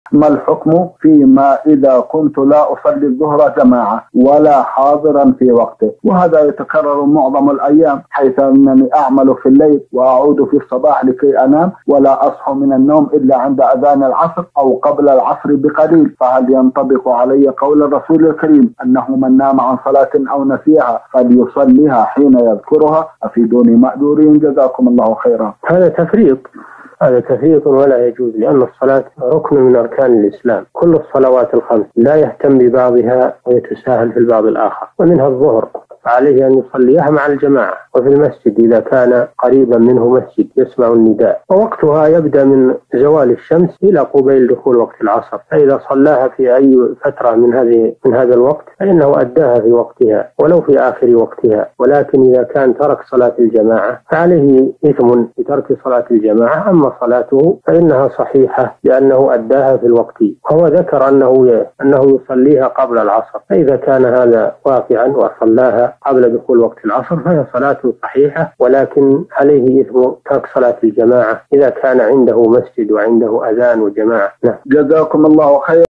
ما الحكم فيما إذا كنت لا أصلي الظهر جماعة حيث أنني أعمل ليلاً وأعود في الصباح وأنام ولا أصحوا حتى..؟
نص السؤال:
ما الحكم فيما إذا كنت لا أصلي الظهر جماعة ولا حاضراً في وقته وهذا
يتكرر في معظم الأيام حيث أنني أعمل في الليل وأعود في الصباح لكي
أنام ولا أصحو من النوم إلا عند أذان العصر أو قبل العصر بقليل فهل
ينطبق علي قول الرسول الكريم أنه من نام عن صلاة أو نسيها فليصليها
حين يذكرها أفيدوني مأجورين جزاكم الله خيراً؟
السيرة الذاتية :
للاشتراك في قناة الشيخ صالح الفوزان

رئيس الفريق
لمشروع كبار العلماء